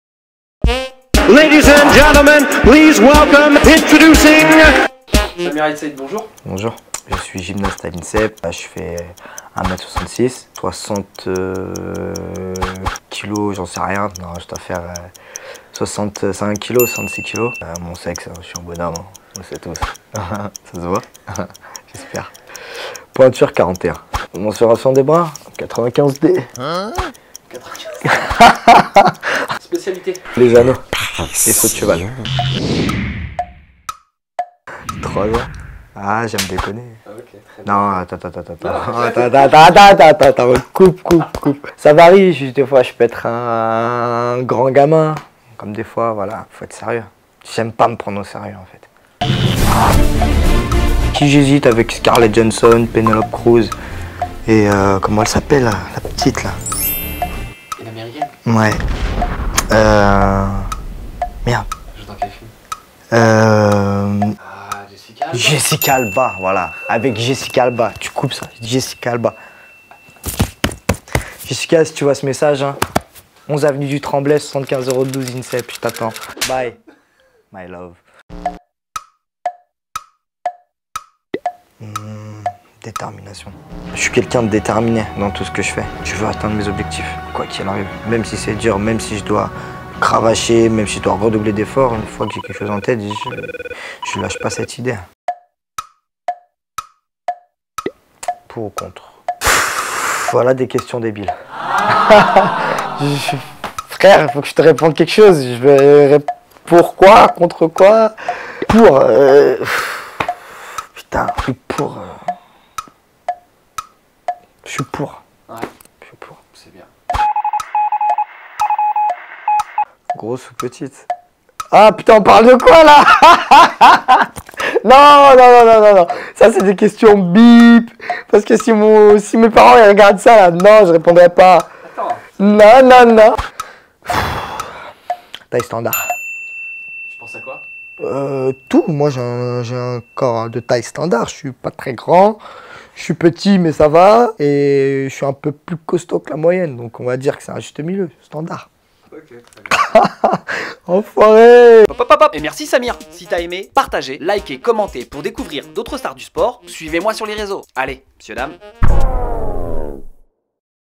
PPPS Samir aït said gymnastique
L'interview décalé de Samir Ait Said ! Gymnaste français, grand espoir de médaille olympique !